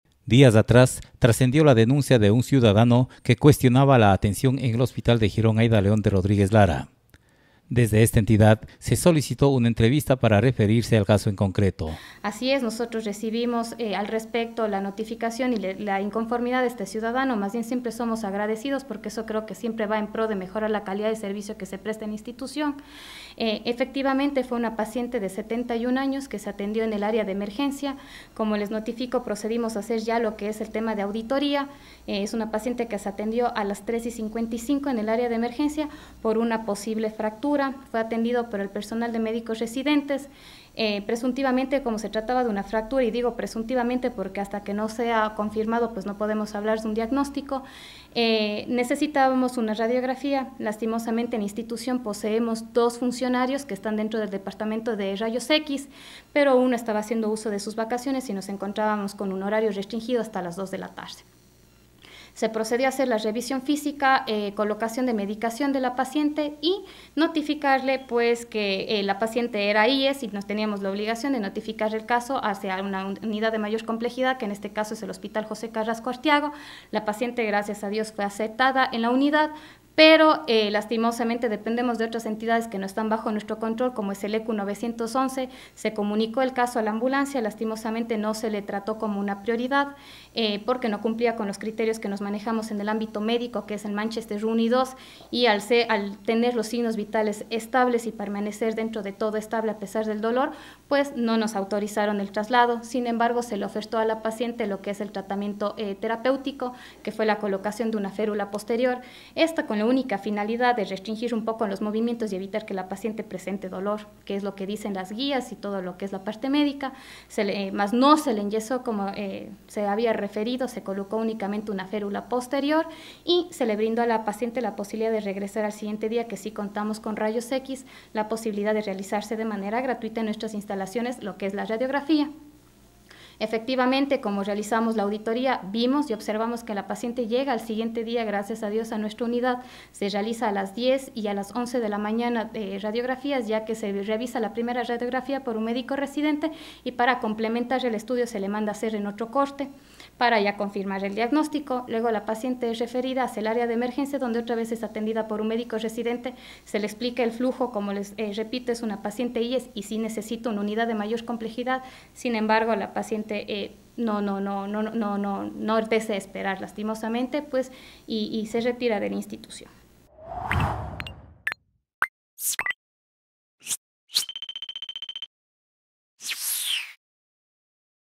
Directora del hospital de Girón aclara situación respecto a una denuncia ciudadana
La semana pasada a través de este medio se difundió el malestar de ciudadano gironense domiciliado en EEUU, por una supuesta mala atención; en aquella nota se presentó un audio de la directora hospitalaria quien dio sus argumentos, sin embargo, se nos ha solicitado una entrevista para aclarar la posición de la casa de salud a través de su directora Dra. María Andrea Sánchez frente a lo acontecido.